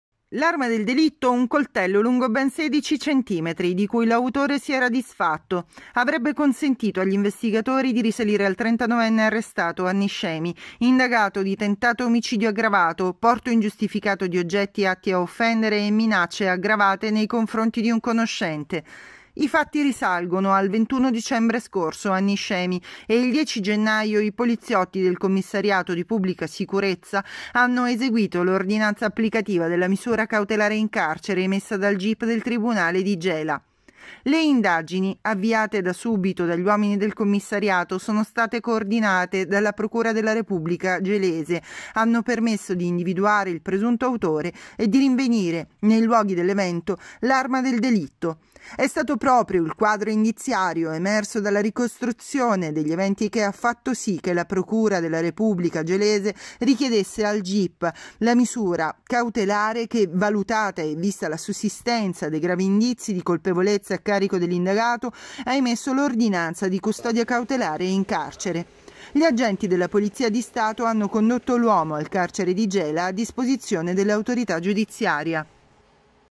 Niscemi - Tentato omicidio, arrestato un niscemese
L’uomo è accusato anche di minacce e porto di coltello